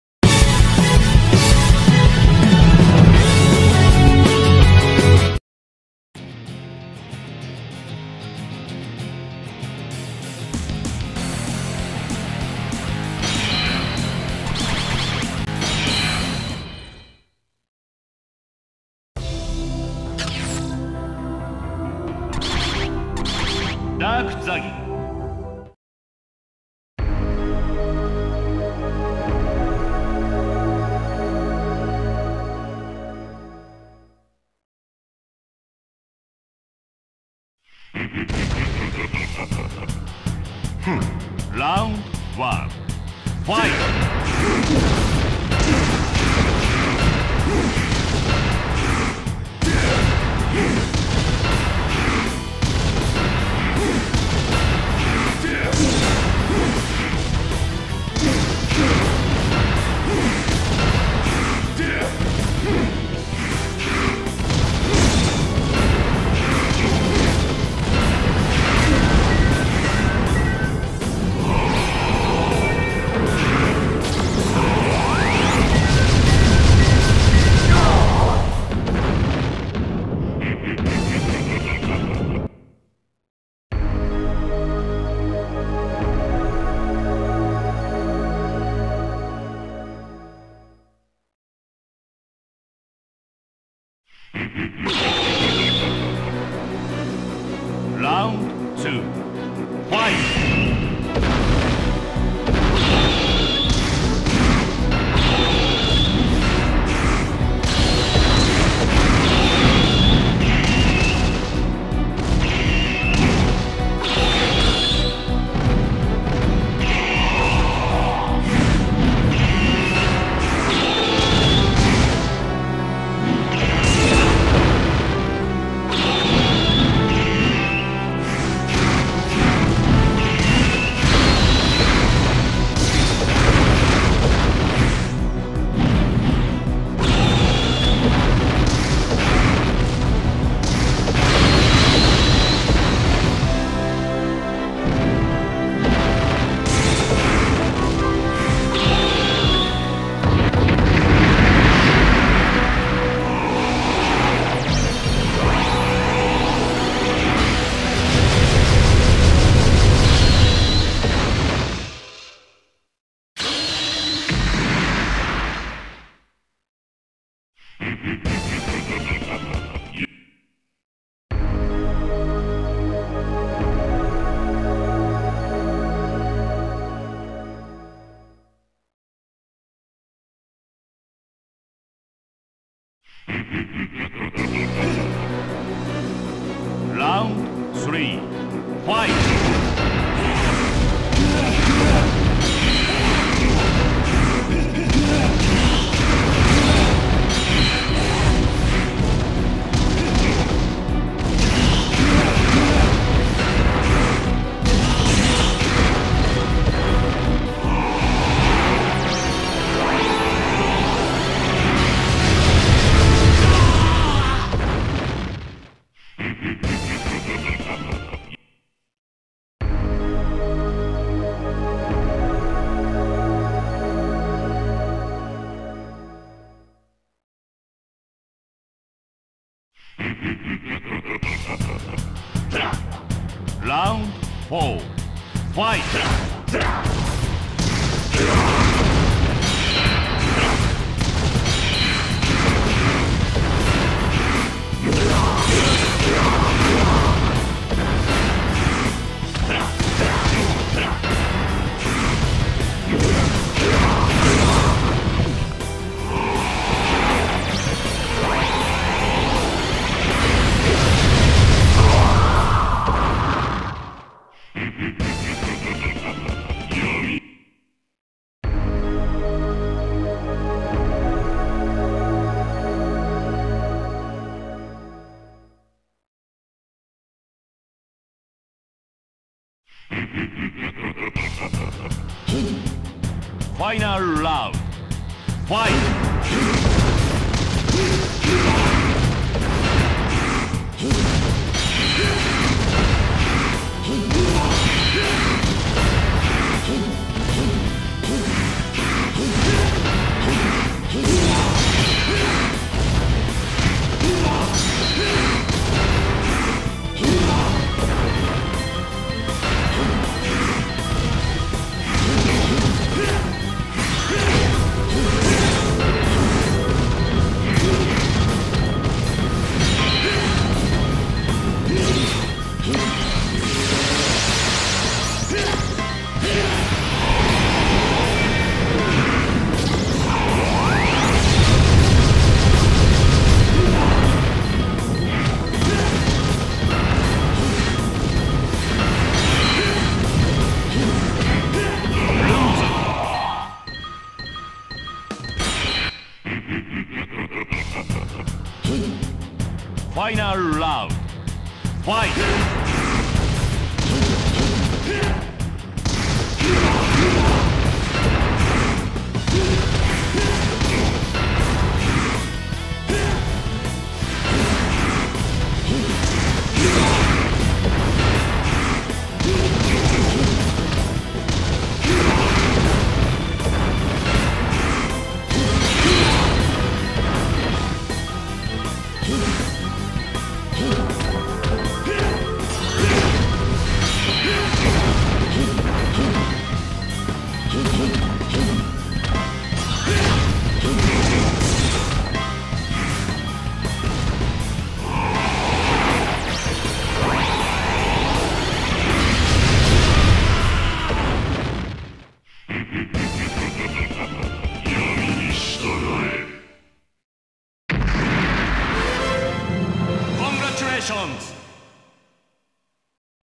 BATTLE MODE DARK ZAGI BATTLE EPIC PARAH!!!
Oke temen temen kali ini kita akan bermain battle mode menggunakan DARK ZAGI